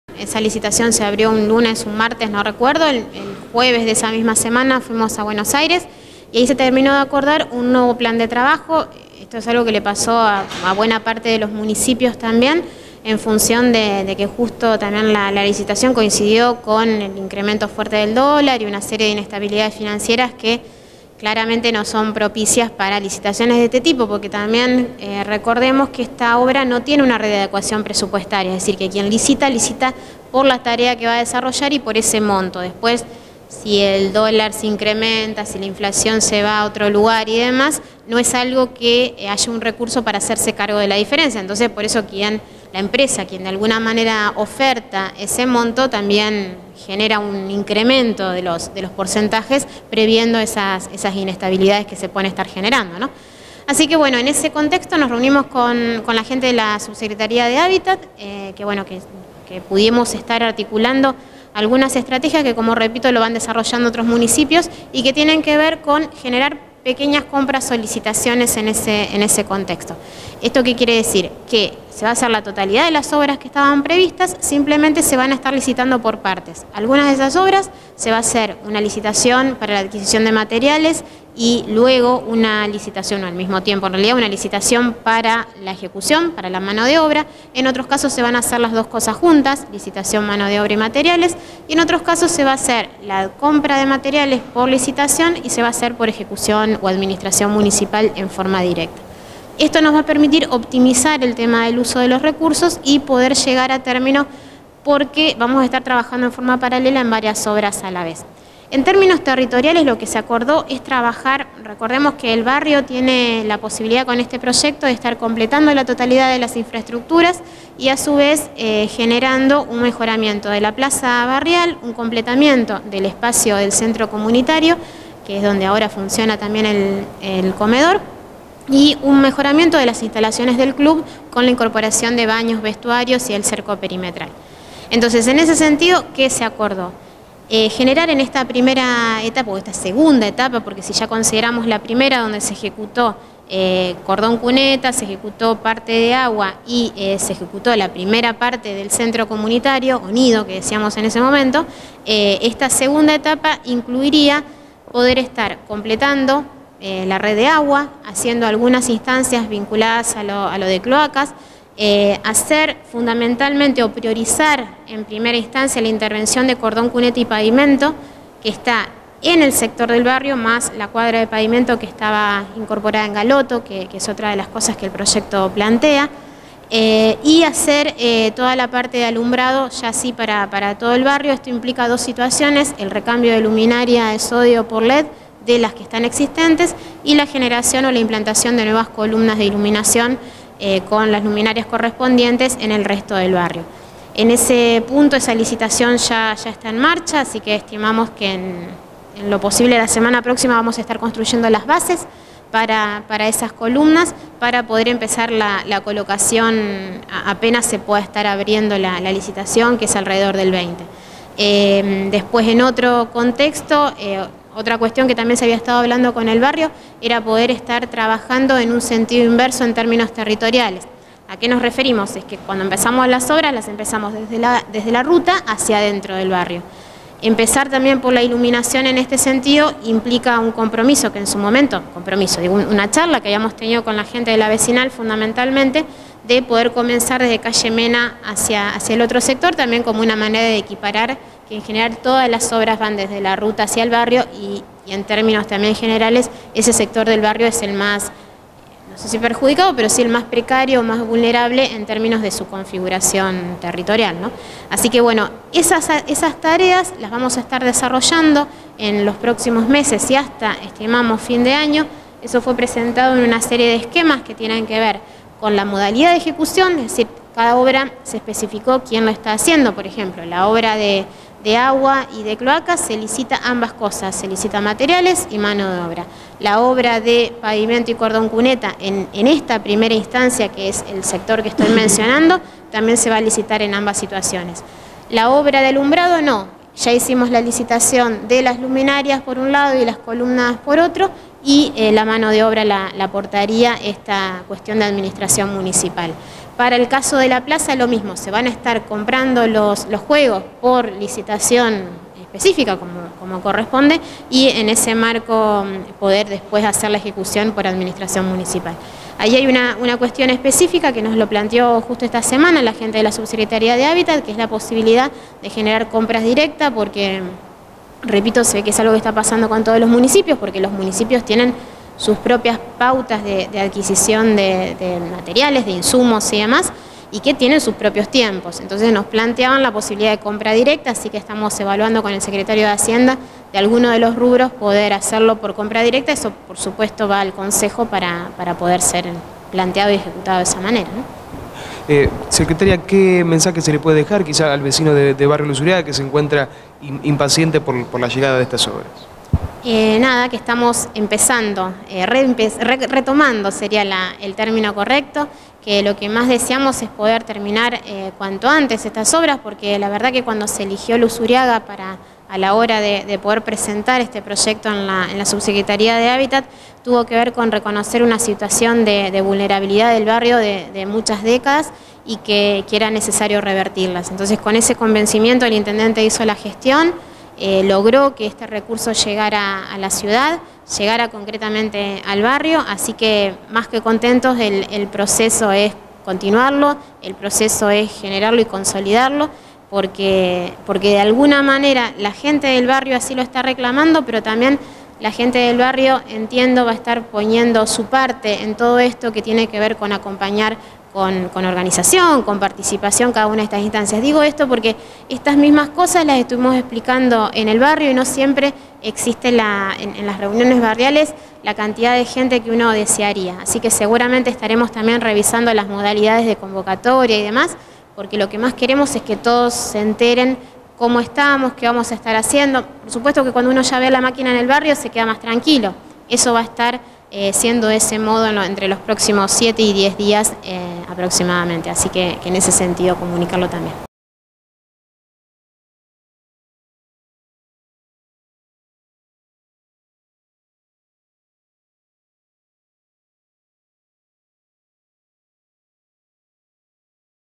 PAOLA BAGNERA, INFORMÓ SOBRE LA SITUACIÓN DE LAS OBRAS EN BARRIO LUZURIAGA
PAOLA BAGNERA; SEC  ORDENAMIENTO TERRITORIAL